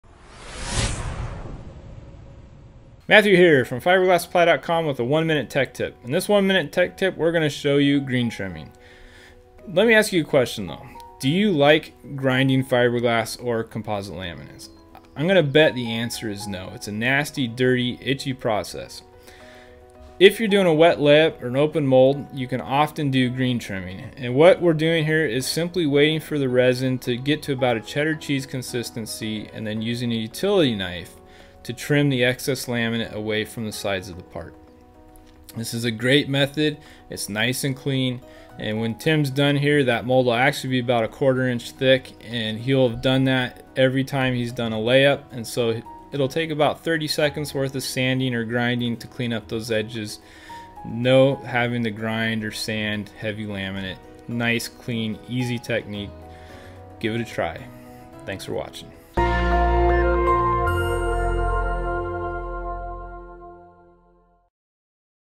One Minute Tech Tip: Green Trimming Fiberglass Laminate
In this video we'll show you a quick tip for trimming fiberglass or composite parts that are built using open molding techniques that will save you time and frustration.

Green trimming is simply trimming the laminate with a utility knife when the resin is cured to about the state of cheddar cheese.  When the resin is in this state it is firm enough to cut without having the fibers pull out and soft enough that a sharp blade can easily cut through it.  This method does not create any dust and can make a very clean trim line, reducing or sometimes eliminating the need to grind after the part is cured.